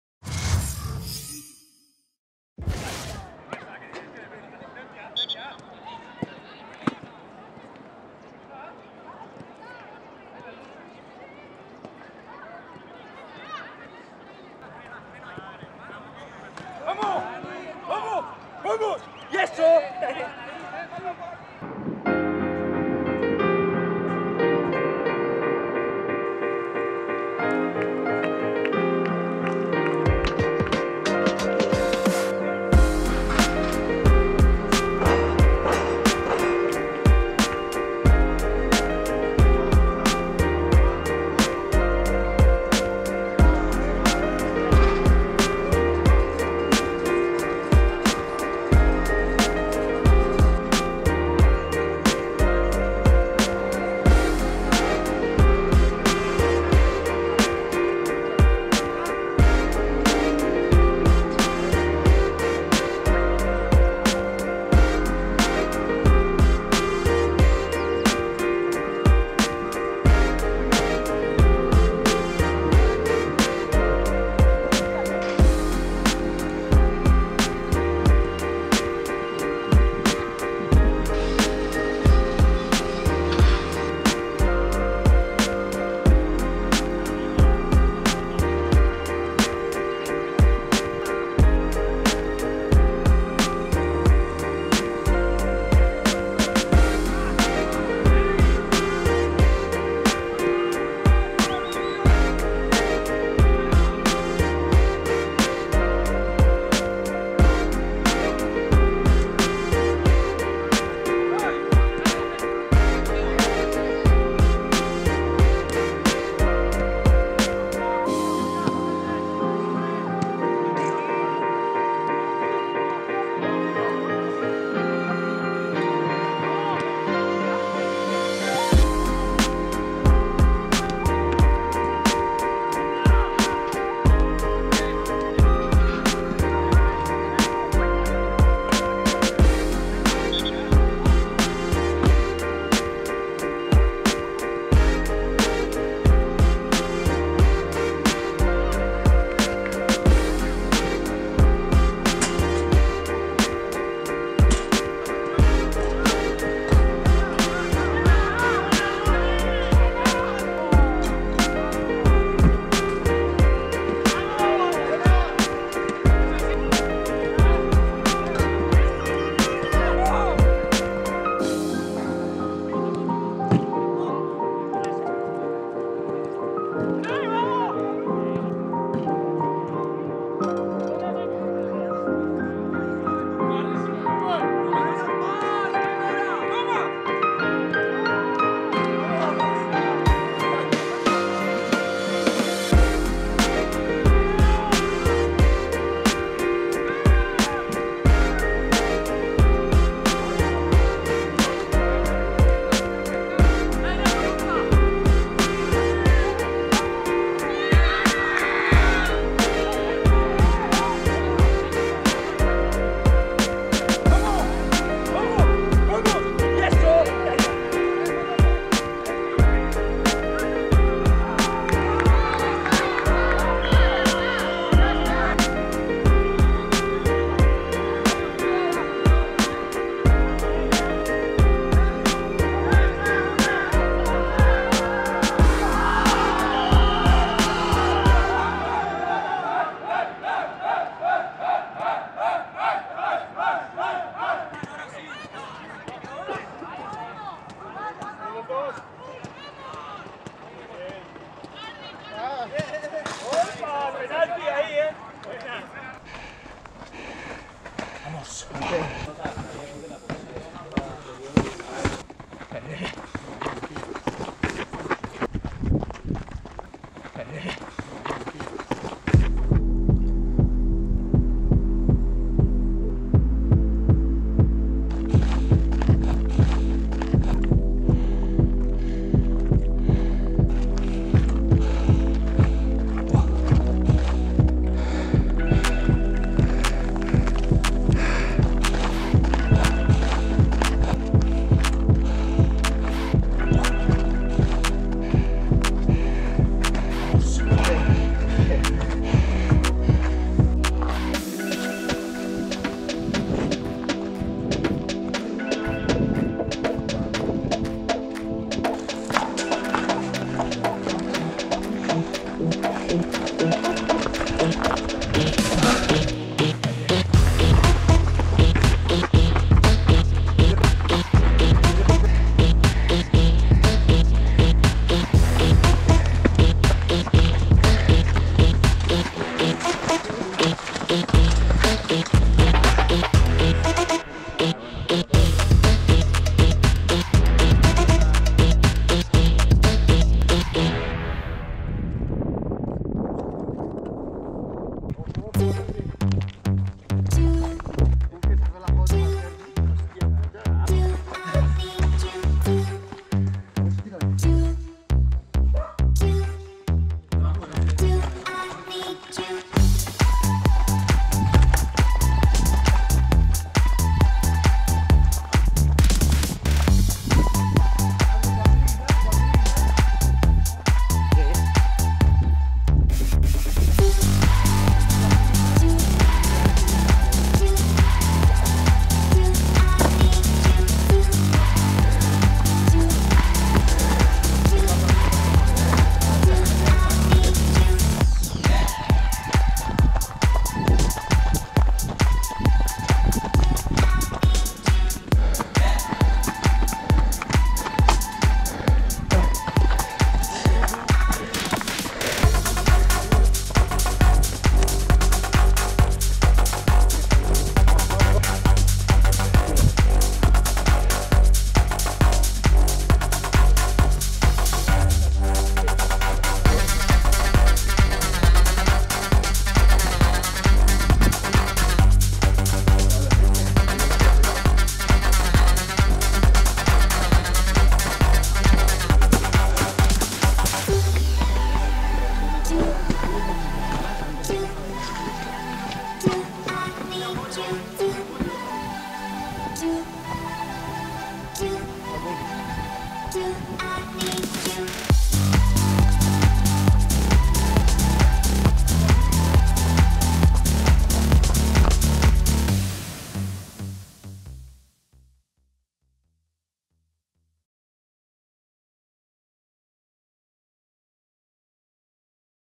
FIRST TEAM BACK IN TRAINING + SERGI ROBERTO & WAGUE KINGS OF THE MOUNTAIN 💪💪
Today saw Xavi and the squad return to the Ciutat Esportiva. Smiles everywhere, but they trained hard without and without the ball with the game against Elche in mind. The Barça players will definitely be targeting continuing their winning run.

It was a different kind of training day for Sergi Roberto and Moussa Wague. The two players, who are still recovering from their respective injuries, shared a session away from the Ciutat Esportiva, the gym and the training ground.